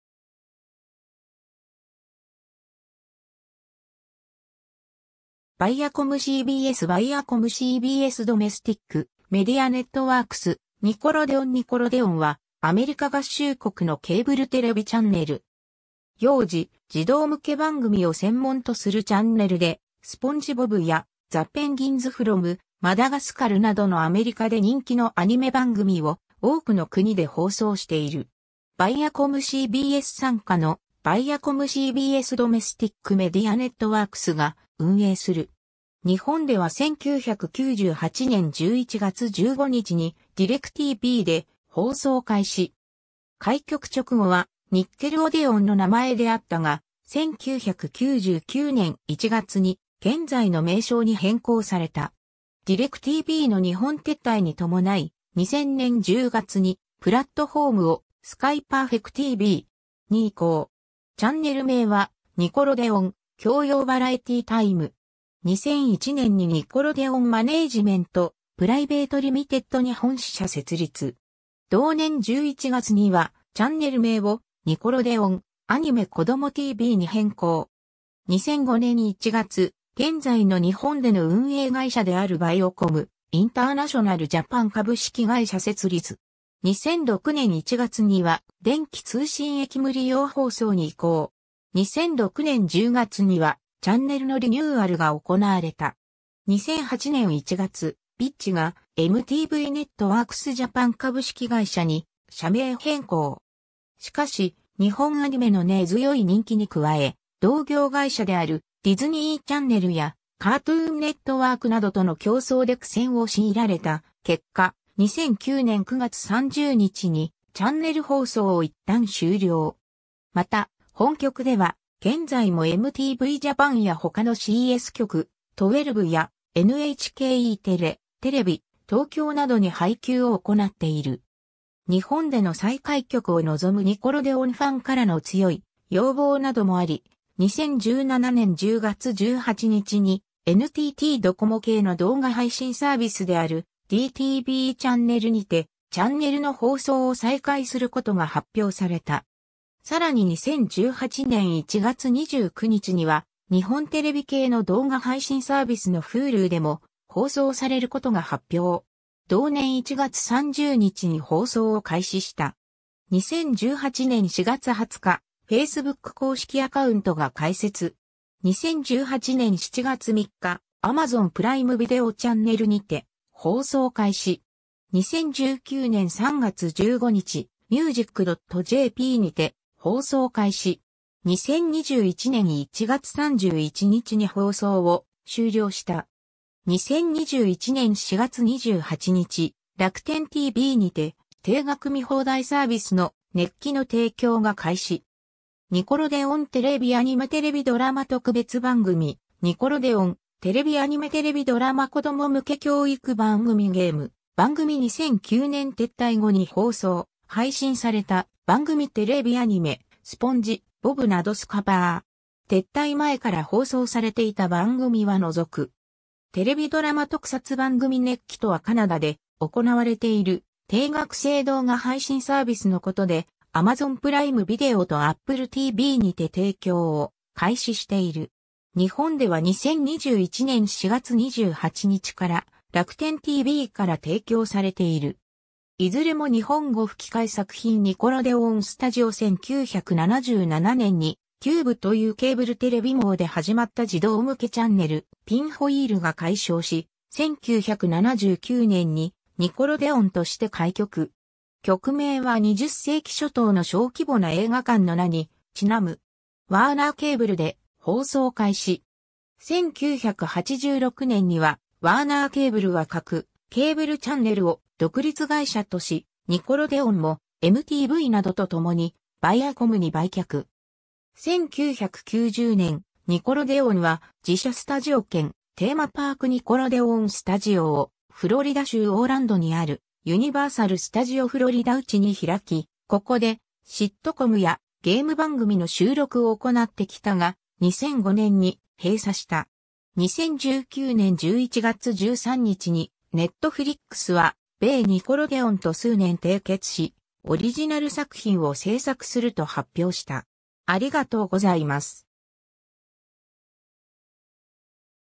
ニコロデオン (TVチャンネル)
かつて存在したスカパー!のテレビチャンネル
バイアコムCBS 」 バイアコムCBSドメスティック・メディア・ネットワークス 」 ニコロデオン (TVチャンネル) ニコロデオン（英語: Nickelodeon）は、アメリカ合衆国のケーブルテレビチャンネル。
幼児・児童向け番組を専門とするチャンネルで、『スポンジ・ボブ』や『ザ・ペンギンズ from マダガスカル』などのアメリカで人気のアニメ番組を多くの国で放送している。
バイアコムCBS傘下のバイアコムCBSドメスティック・メディア・ネットワークスが運営する。
 日本では1998年11月15日にディレクTVで放送開始（委託放送事業者はワンダーウェーブ株式会社、Ch.272）。
開局直後は「ニッケルオデオン」の名前であったが、1999年1月に現在の名称に変更された。
 ディレクTVの日本撤退に伴い、2000年10月にプラットフォームをスカイパーフェクTV!に移行（委託放送事業者はジャパンイメージコミュニケーションズ、Ch.278）。
チャンネル名は「ニコロデオン/教養バラエティータイム」。
2001年にニコロデオン・マネージメント・プライベート・Ltd日本支社（ニコロデオン・ジャパン）設立。
同年11月にはチャンネル名を「ニコロデオン/アニメ・子どもTV」に変更。
 2005年1月、現在の日本での運営会社であるViacom International Japan（VIJ）株式会社設立。
2006年1月には電気通信役務利用放送に移行（電気通信役務利用放送事業者はMTVジャパン株式会社、現在のバイアコム・ネットワークス・ジャパン株式会社、Ch.751に変更）。
2006年10月にはチャンネルのリニューアルが行われた。
2008年1月、VIJがMTV Networks Japan株式会社（MTVNJ）に社名変更（のち合同会社に組織変更）。
 しかし、日本アニメの根強い人気に加え、同業会社であるディズニー・チャンネルやカートゥーン ネットワークなどとの競争で苦戦を強いられた結果、2009年9月30日にチャンネル放送を一旦終了。
また、本局では現在もMTVジャパンや他のCS局、TwellVやNHK Eテレ、テレビ東京などに配給を行っている。
 日本での再開局を望むニコロデオンファンからの強い要望などもあり、2017年10月18日にNTTドコモ系の動画配信サービスであるdTVチャンネルにてチャンネルの放送を再開する事が発表された。
更に2018年1月29日には日本テレビ系の動画配信サービスのHuluでも放送される事が発表。
同年1月30日に放送を開始した。
 2018年4月20日、Facebook公式アカウントが開設。
 2018年7月3日、Amazonプライム・ビデオチャンネルにて放送開始。
 2019年3月15日、music.jp（テレビコース）にて放送開始。
2021年1月31日に放送を終了した。
 2021年4月28日、楽天TVにて定額見放題サービスの『nick+』（ニック・プラス）の提供が開始。
 ニコロデオン（2018年 - ） テレビアニメ テレビドラマ 特別番組 ニコロデオン（1998年 - 2009年） テレビアニメ テレビドラマ 子供向け教育番組 ゲーム番組 2009年撤退後に放送・配信された番組 テレビアニメ 『スポンジ・ボブ』などスカパー!撤退前から放送されていた番組は除く。
 テレビドラマ 特撮番組 nick+(ニックプラス)とはカナダで行われている定額制動画配信サービスのことで、Amazon Prime VideoとApple TVにて提供を開始している。
日本では2021年4月28日からRakuten TVから提供されている。
 いずれも日本語吹き替え作品 ニコロデオン・スタジオ 1977年に「QUBE」というケーブルテレビ網で始まった児童向けチャンネル「ピンホイール」が改称し、1979年に「ニコロデオン」として開局。
局名は20世紀初頭の小規模な映画館の名にちなむ。
ワーナー・ケーブルで放送開始。
1986年にはワーナー・ケーブルは各ケーブルチャンネルを独立会社とし、ニコロデオンもMTVなどとともにバイアコムに売却。
 1990年、ニコロデオンは自社スタジオ兼テーマパークニコロデオン・スタジオをフロリダ州...